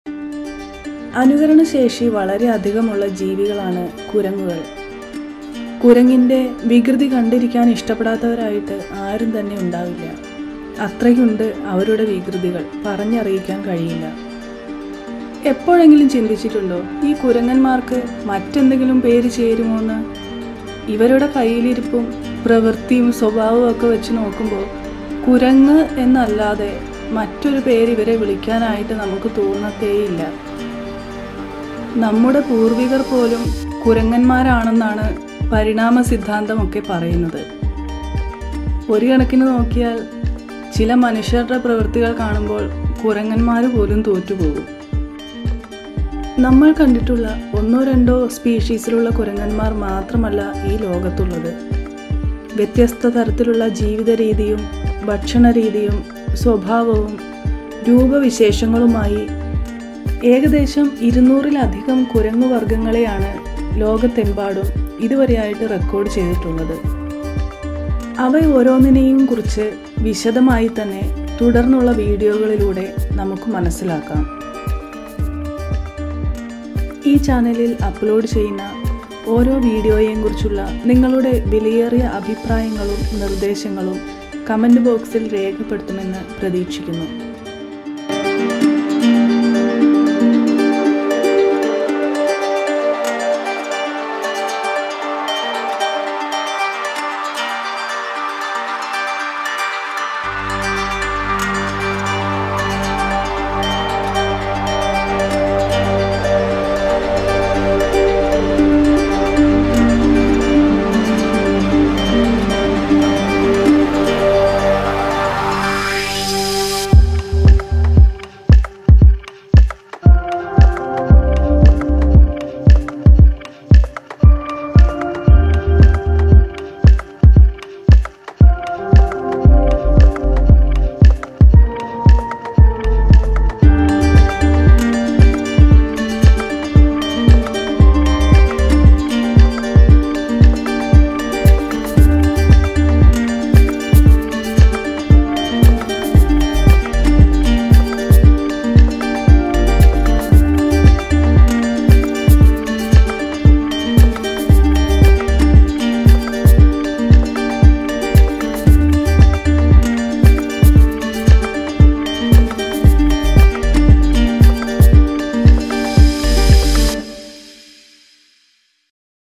ഒരു കുരങ്ങ് ചരിതം | Trailer Video | wildlife documentary
'🐒 Get ready for an exciting adventure with our primate friends in the latest vlog from Childs of Wild! 🌍 Dive into the captivating world of monkeys, as we explore their unique behaviors, incredible adaptations, and the sheer joy they bring to our lives. 🎥 This trailer gives you a sneak peek of what's to come, so don't miss out! 🚀